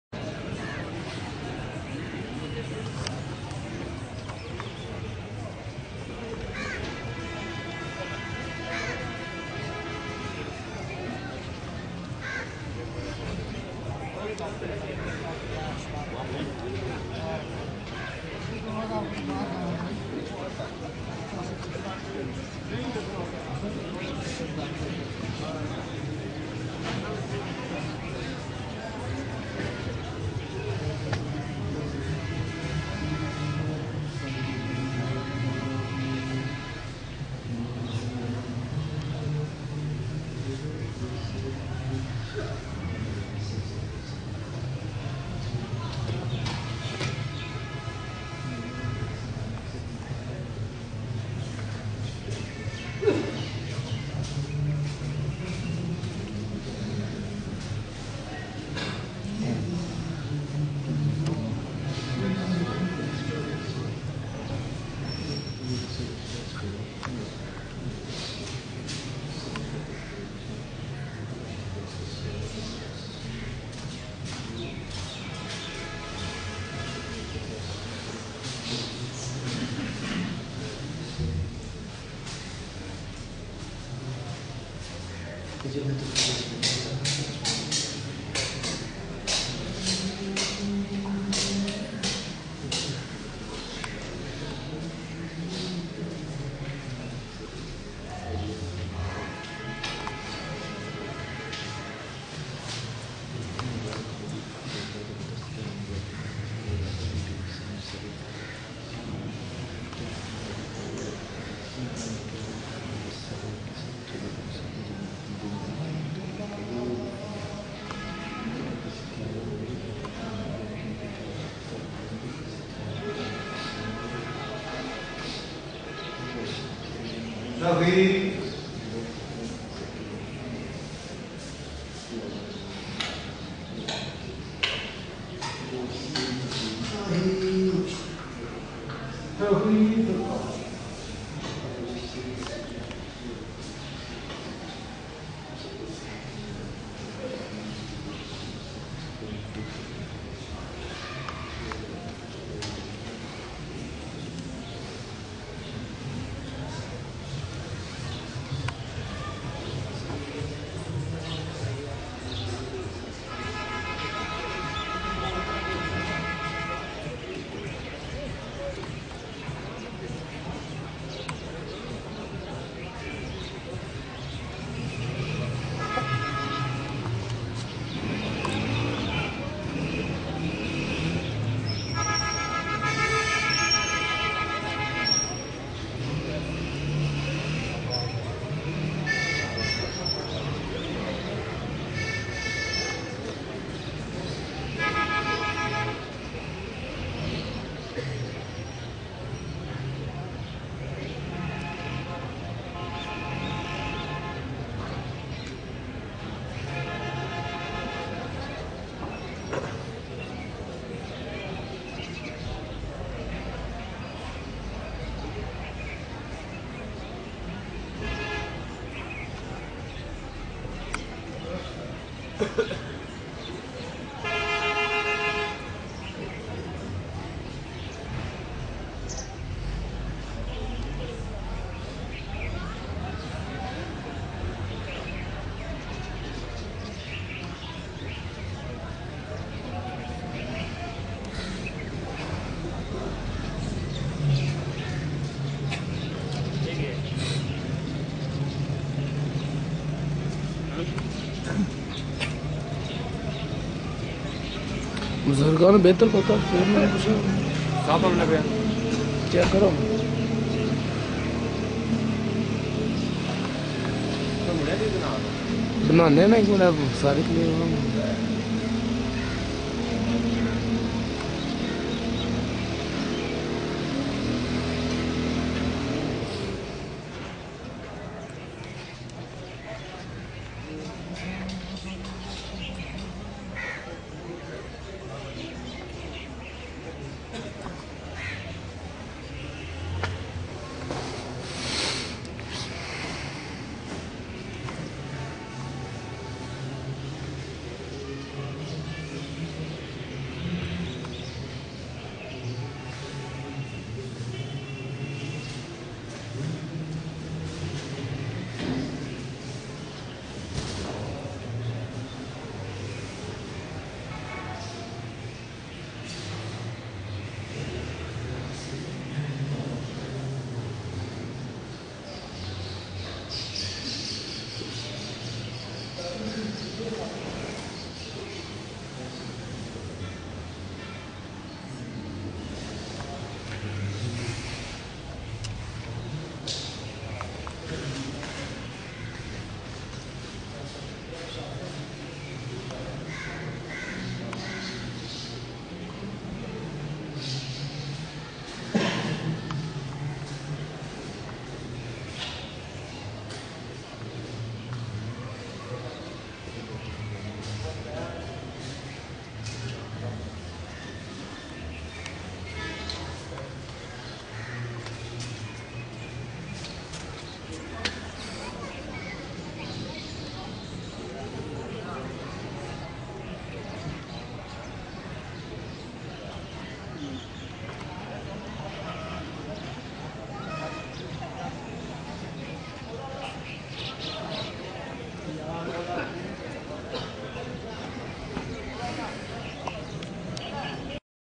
Bawli 1
Shahzad, Awais Visit The Wadi Neelam from Village Bawli , Sarai Alamgir.